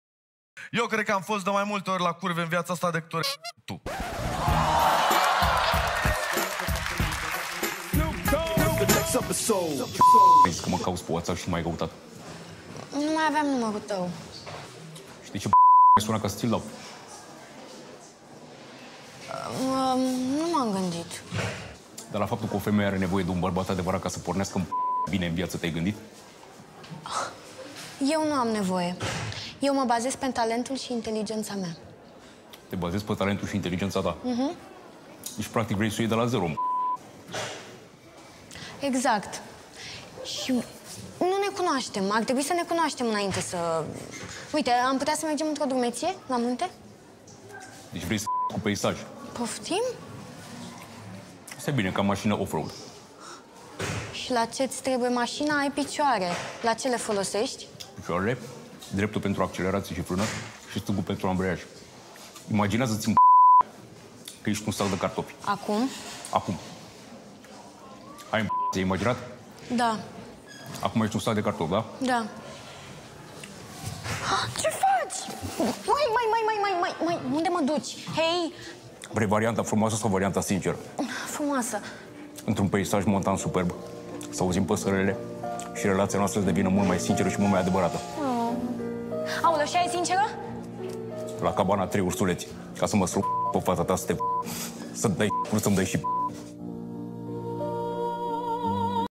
Cheloo merge la agățat! Din păcate sinceritatea nu îl ajută, dar întodeauna există varianta răpirii
Cheloo este hotărât să agațe și el ceva. Cel mai bun loc se află în facultățile din România. Artistul se foloseste de sinceritatea lui debordantă, dar din păcate tânăra nu îl ia seamă.